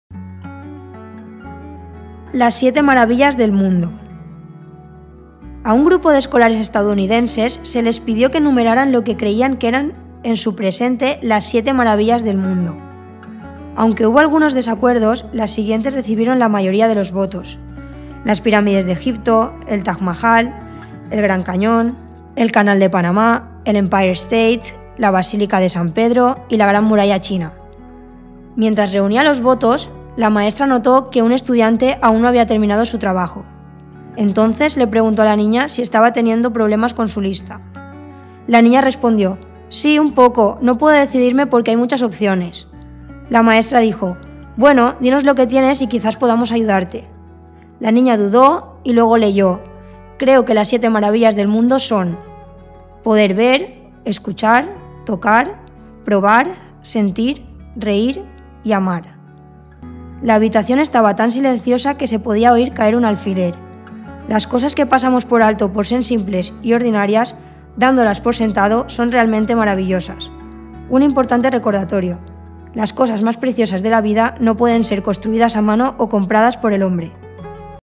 Las siete Maravillas
Resumen:
Una profesora preguntó a sus alumnos ¿cuáles son las Siete Maravillas del mundo? Cada alumno se esforzó por localizarlas lo más rápido posible, pero una niña pensó en elegir solamente las que consideraba verdaderamente maravillosas. Cuando la profesora le preguntó cuál era su respuesta final, dijo: "Creo que las "Siete maravillas del mundo" son: Poder ver, Escuchar, Tocar, Probar, Sentir, Reir y Amar”

Resultados de aprendizaje:
Esta historia enseña a los niños a no dar por sentado las cosas simples que hacen que la vida sea mejor y a estar agradecidos por lo que tienen y por la gente que hay en su vida. Por otra parte también sirve para aprender a dar las gracias y a mostrar aprecio.